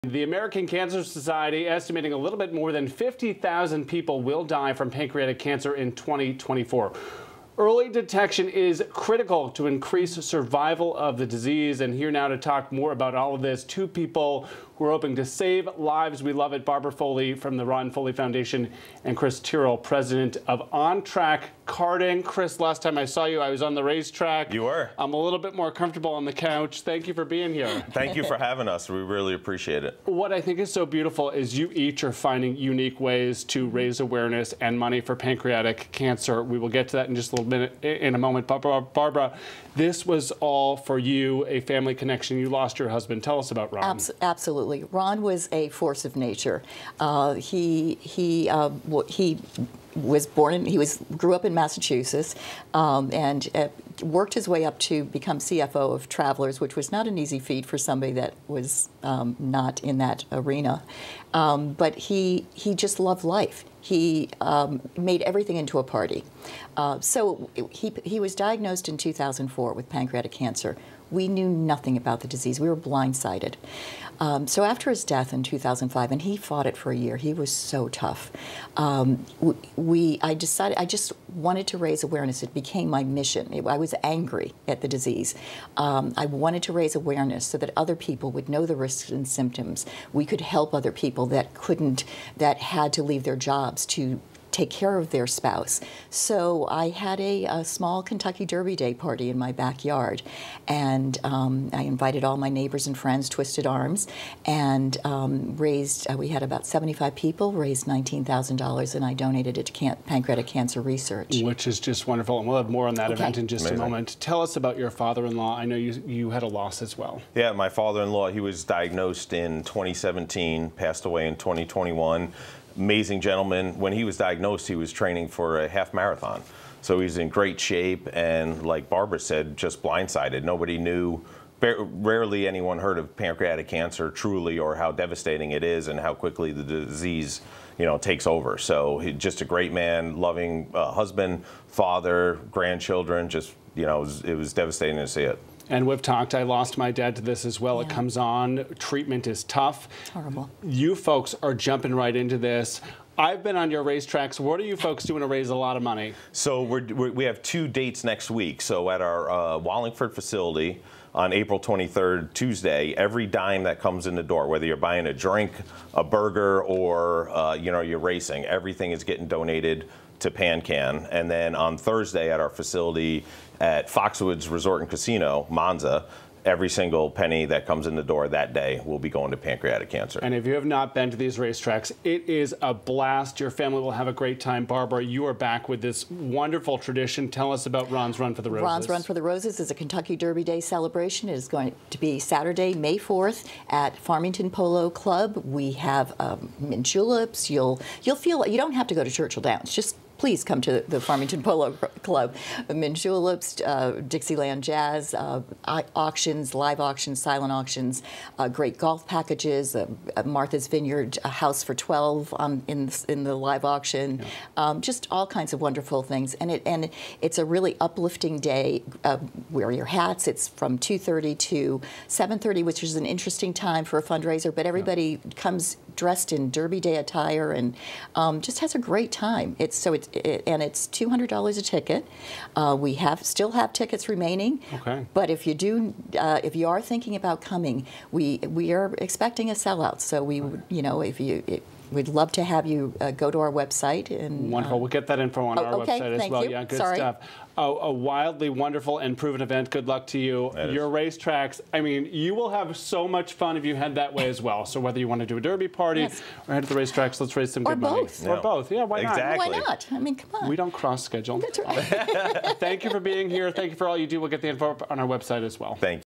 Go-Karting fundraiser and 'Ron's Run for the Roses' will raise money for Pancreatic Cancer Awareness
On Track Karting’s upcoming go-karting fundraiser and The Ron Foley Foundation’s ‘Ron’s Run for the Roses’ will both raise money for Pancreatic Cancer Awareness.